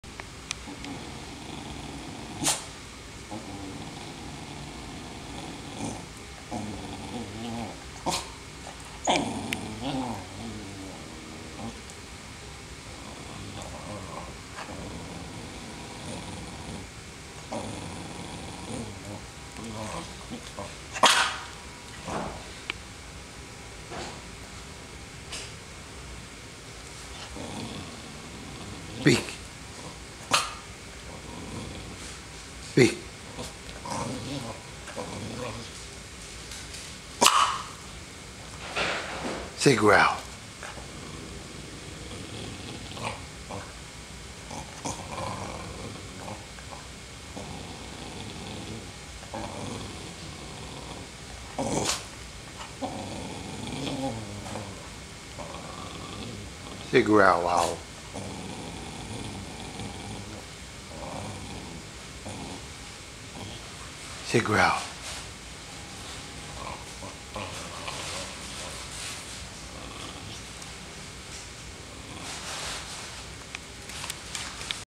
Duke Speaking
Duke speaking and saying growl. He does better when I have a treat. This video was taken with my compact digital camera. The Canon SD780. It takes amazing High Definition video when in full daylight.